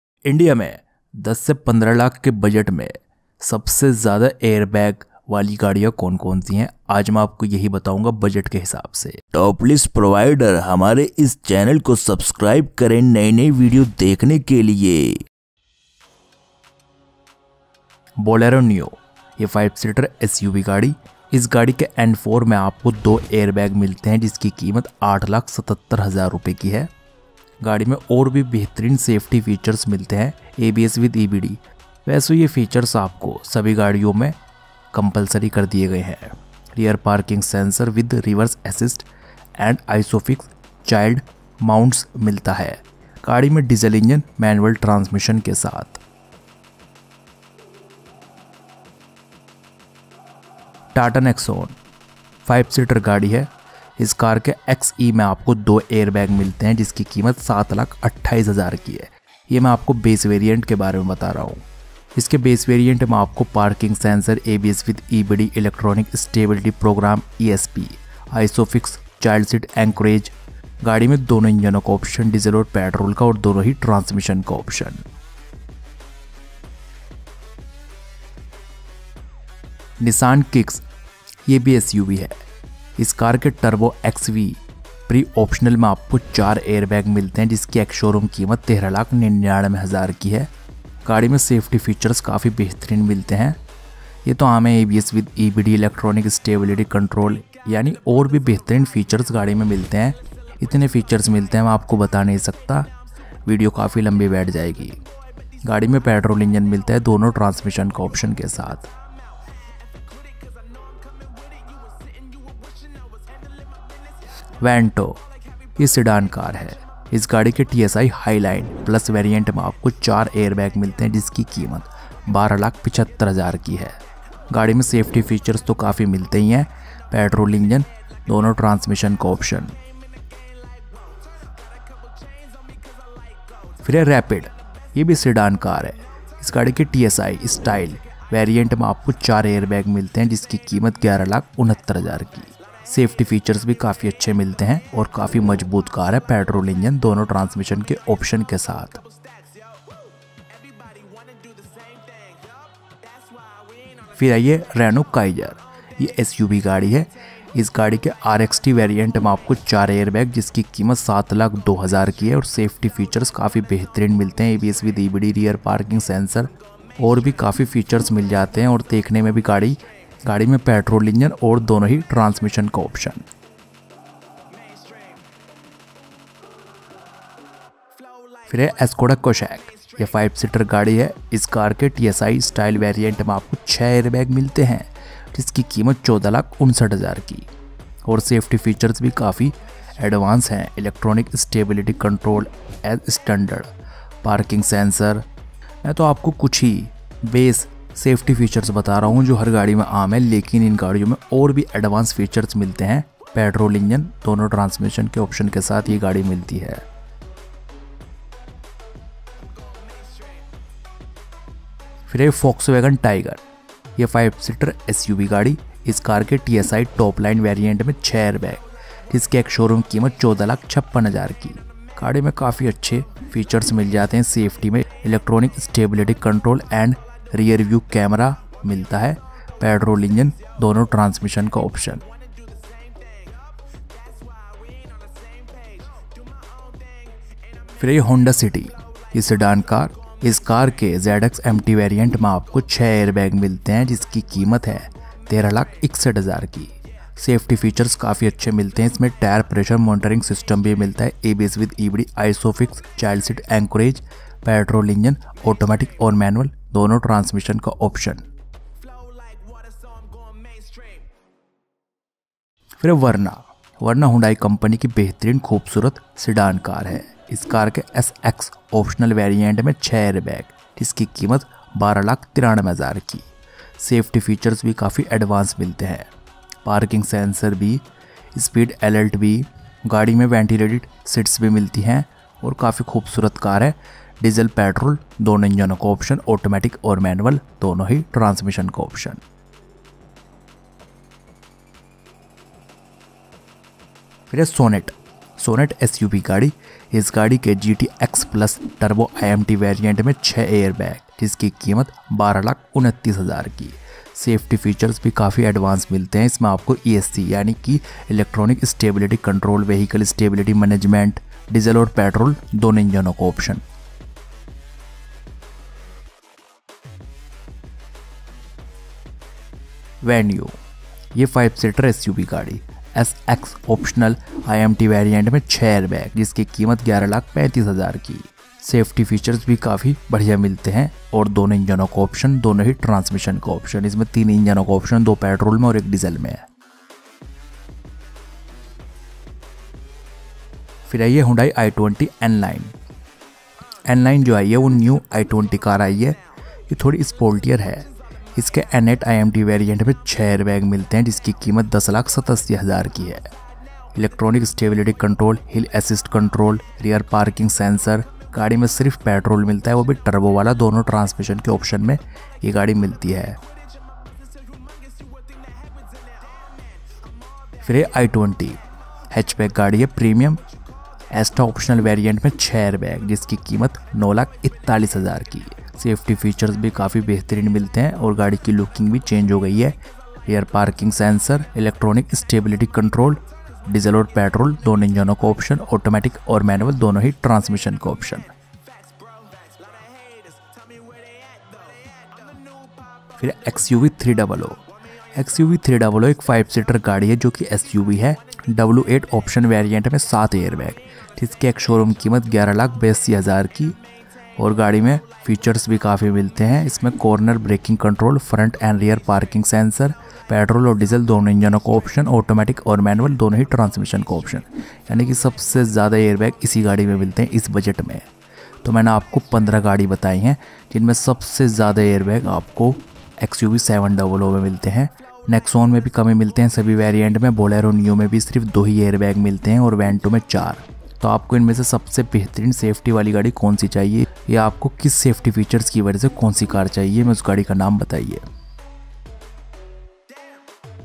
15 लाख के बजट में सबसे ज्यादा AIRBAG वाली गाड़ी
15 लाख के बजट में सबसे ज्यादा AIRBAG वाली गाड़ी

TOP | FIFTEEN | LIST

Most Airbags Cars Under 15 Lakhs

Bolero Neo - Nexon
Kicks      - Vento
Rapid      - Kiger
Kushaq     - Taigun
City       - Verna
Sonet      - Venue
i20 N Line - i20
  Mahindra XUV300